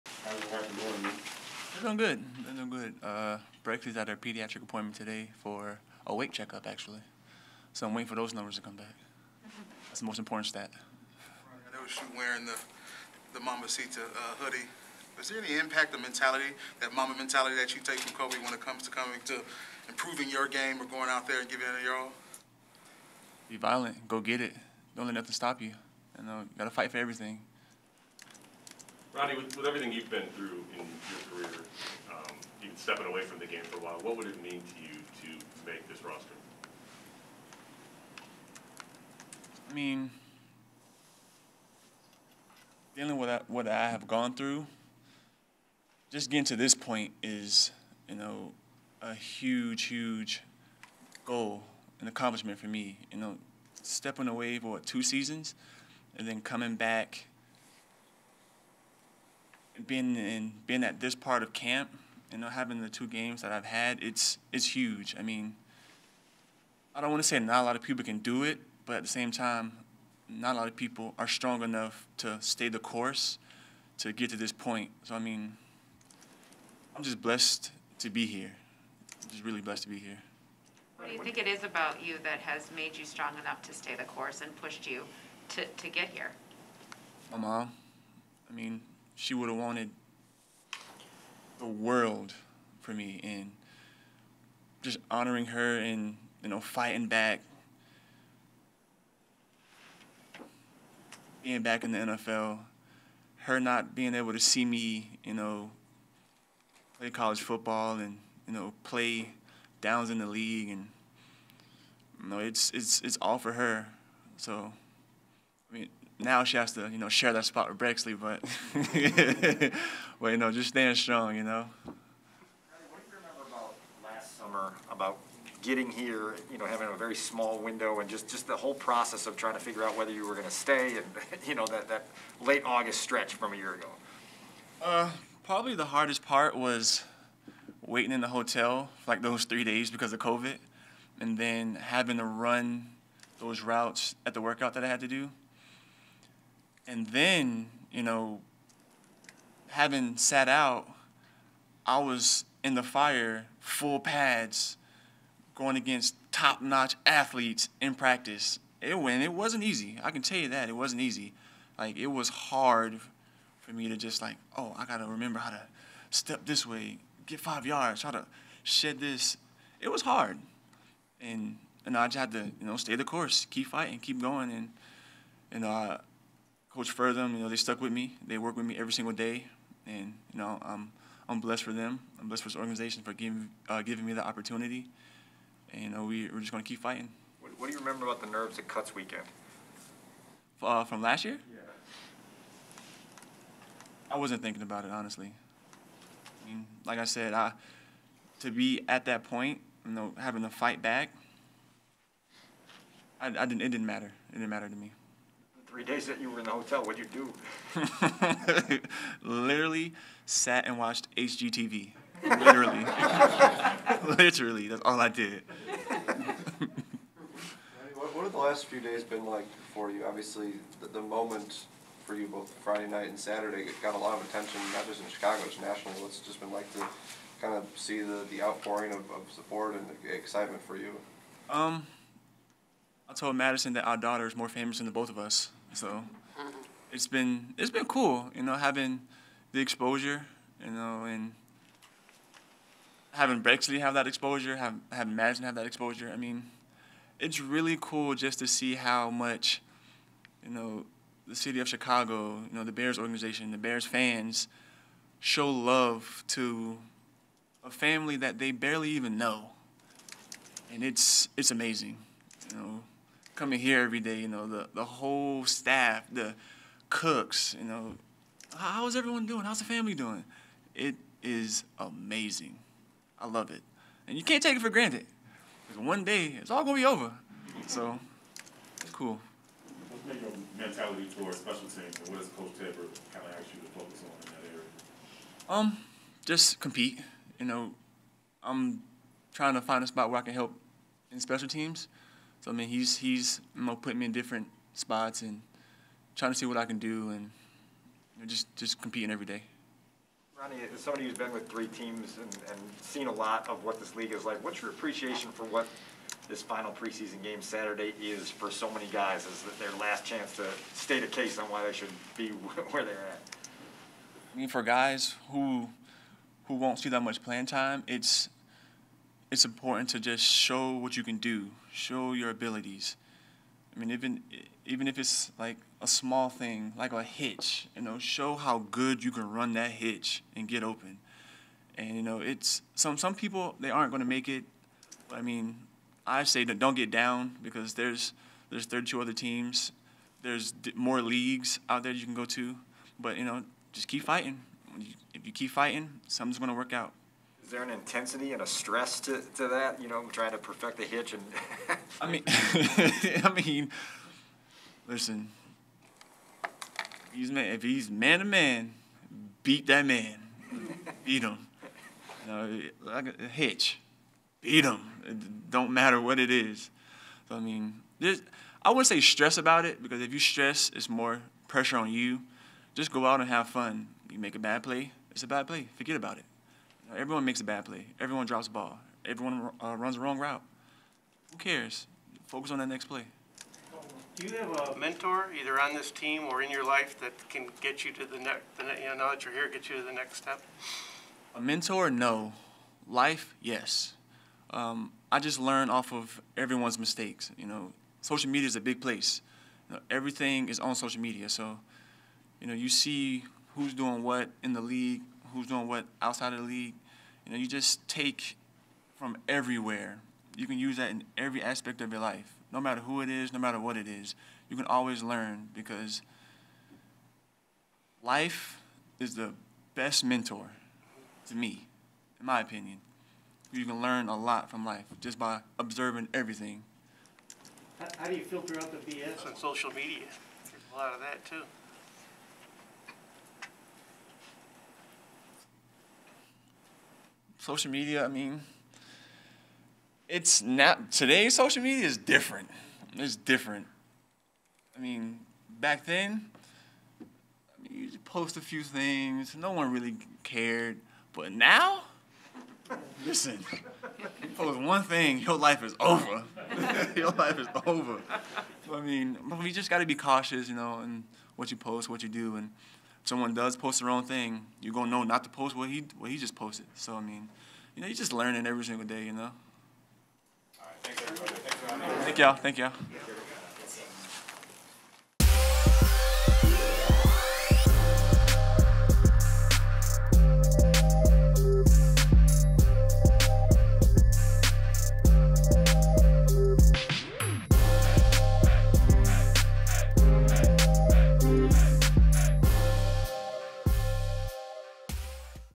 Rodney Adams: 'Life is the best mentor' | Chicago Bears
Wide receiver Rodney Adams addresses the media after Wednesday's training camp practice at Halas Hall.